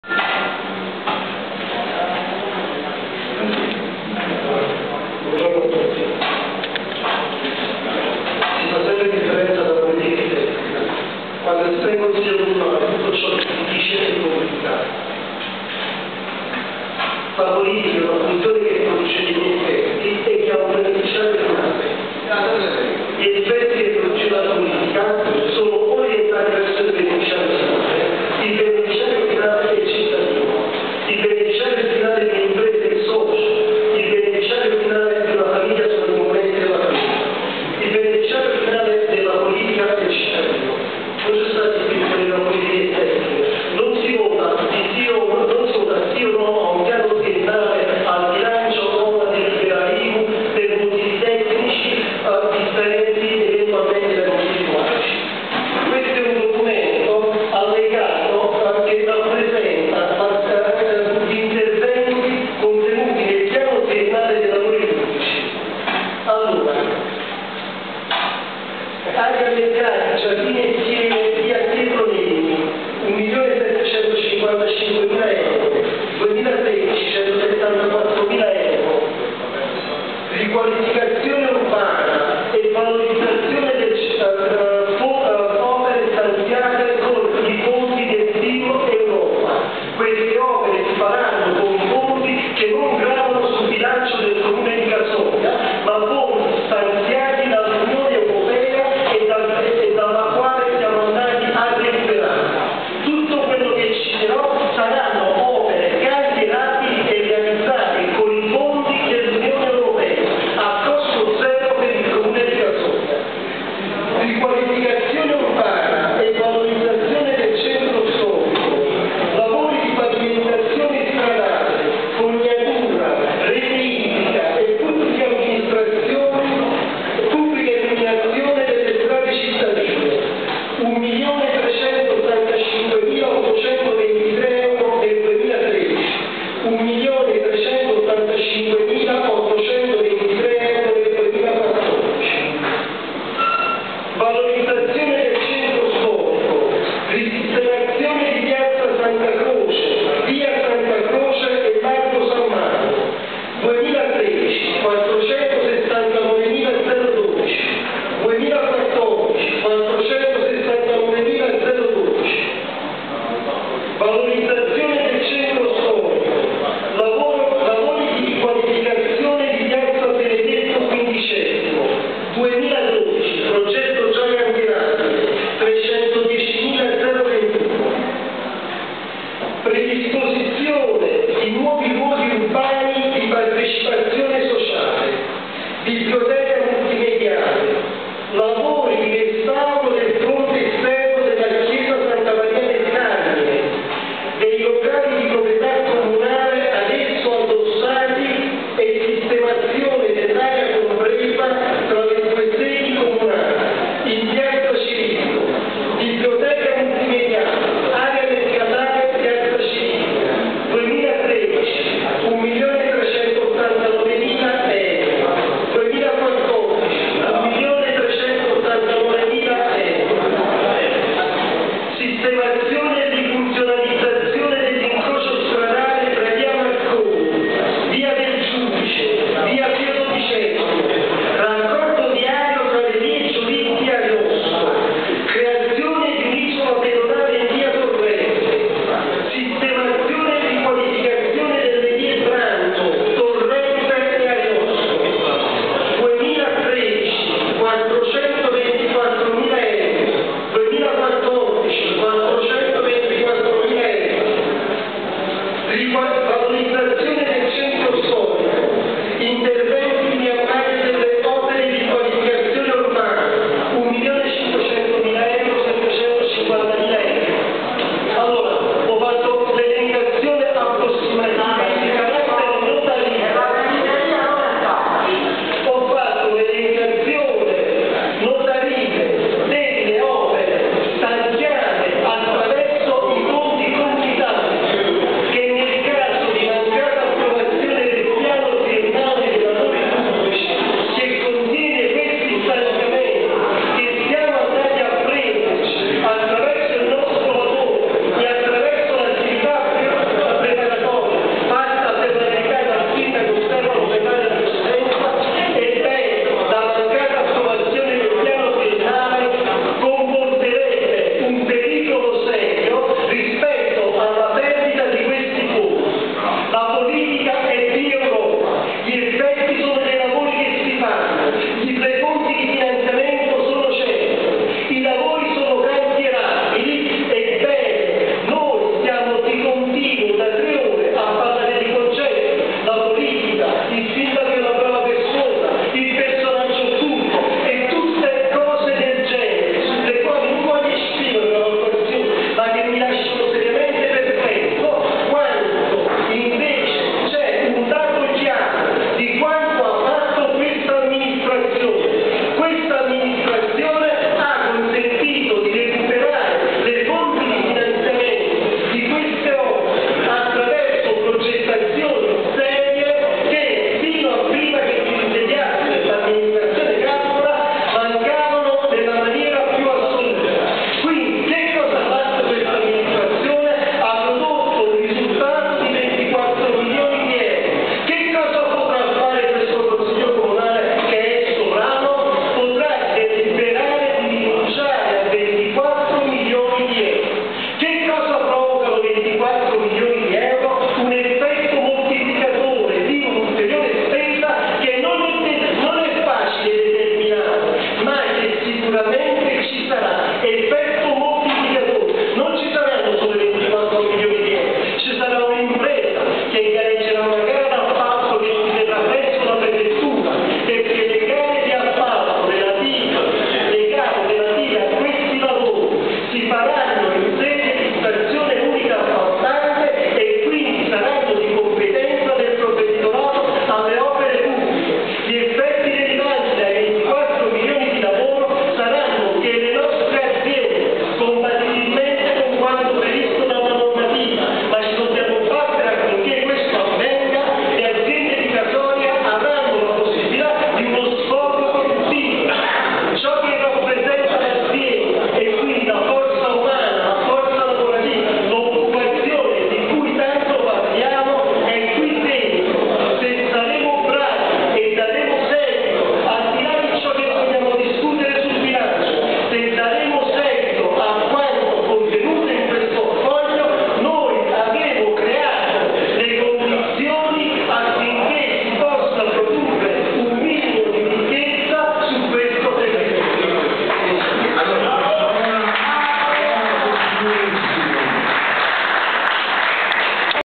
Casoria 18/07/2012 - Consiglio Comunale (19)
"Casoria Ieri & Oggi online" presenta:
Casoria mercoledì 18 luglio 2012, alle ore 09:00 presso l'Aula Consiliare di Piazza Cirillo, la seduta straordinaria del Consiglio Comunale con il seguente Ordine del Giorno:
1) Comunicazioni del Presidente;
2) Determinazione aliquota IMU di competenza comunale anno 2012 -- Proposta al Consiglio omunale;
3) Programma triennale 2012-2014 dei LL.PP. - Elenco annuale redatto secondo schema tipo del decreto ministeriale infrastrutture e trasporti n° 898/IV del 22/06/98 -- Proposta al consiglio;
4) Ricognizione degli immobili di proprietà comunale suscettibili di alienazione e/o valorizzazione, ai sensi dell'art.58 del dl. 112/98, convertito con modificazioni nella legge L:N: 133/98;
5) Bilancio di previsione 2013 -- Bilancio pluriennale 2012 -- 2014 -- Relazione Previsionale e programmatica;
6) Legge regionele n°9/83 -- Delibera d G.R. N° 5447/2002 -- Legge regionale n°16/2004 .
Adozione di variante al PRG vigente per integrazione della N.T.A. alle prescrizioni di cui al parere n°16/2006 espresso dal CTR in data 02/01/2006, intesa come adeguamento alle risultanze della nuova indagine e verifica alla normativa sismica. Adozione ai sensi dell'art. 24 della L.R. N°16/2004.
Intervento del consigliere Biagio Galluccio (Api)